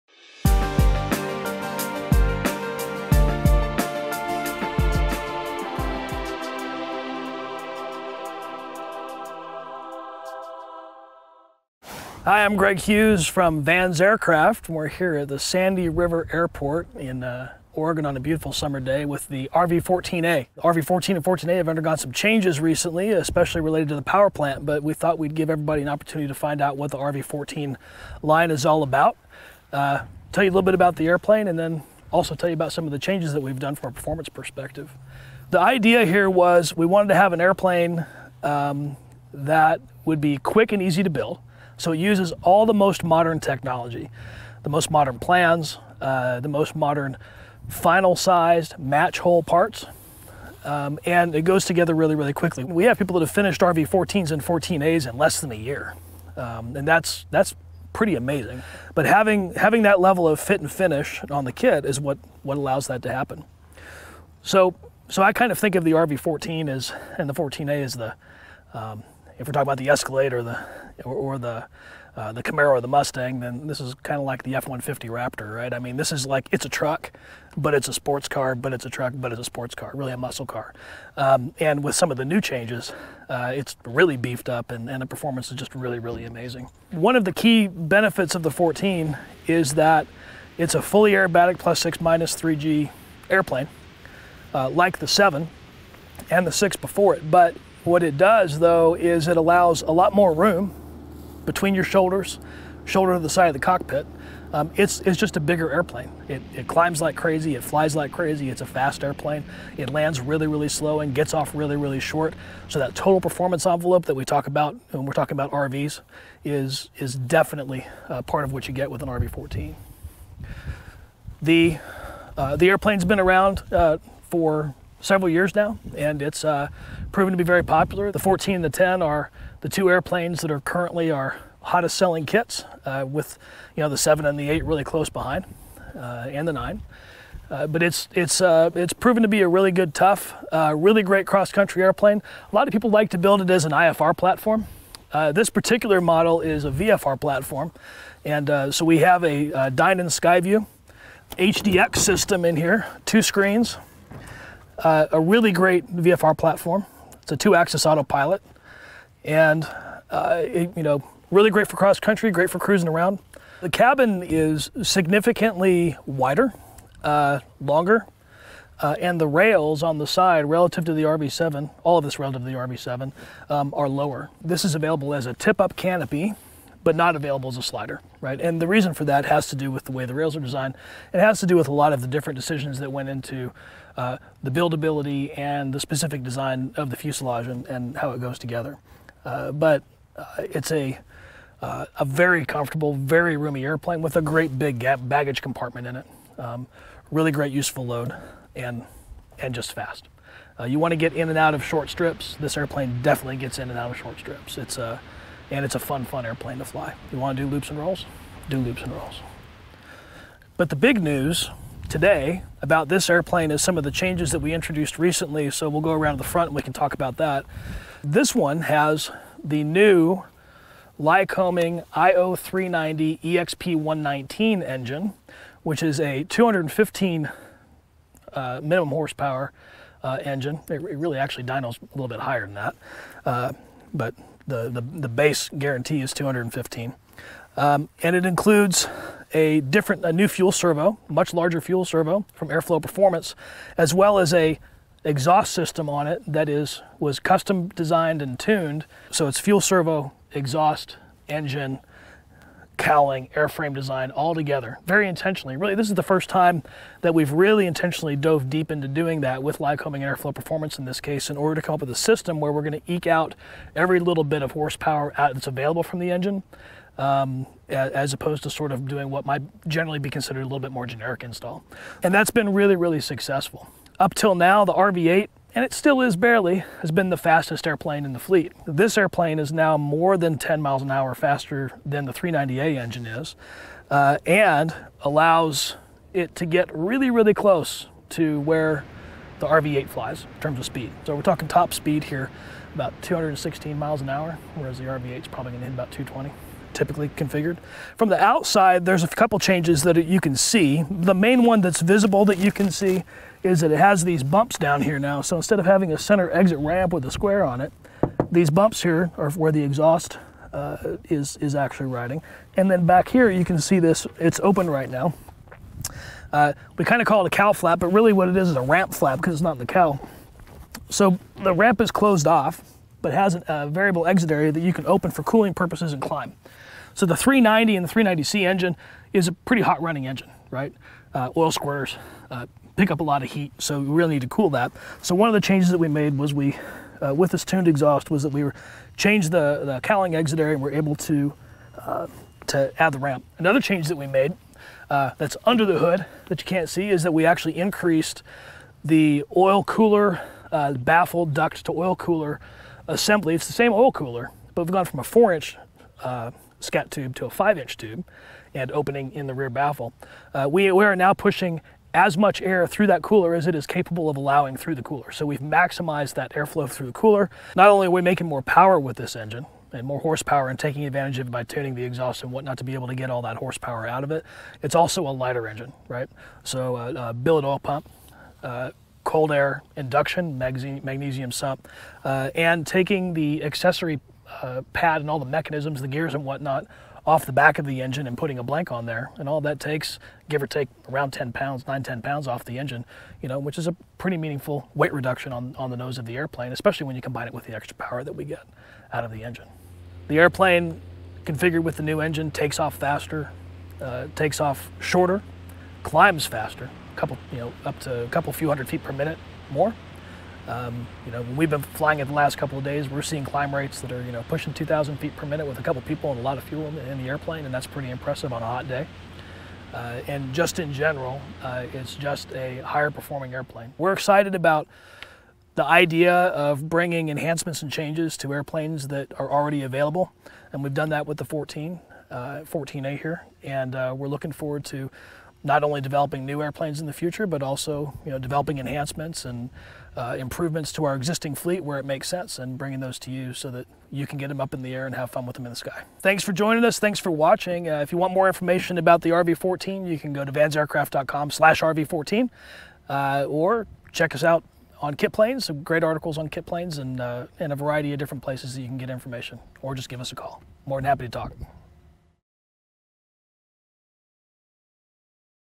Van's Up-Powered RV-14A Walkaround
What's new with the RV-14A? Van's Greg Hughes walks us through the changes for the '14 and the updates surrounding the Lycoming IO-390-EXP119 engine.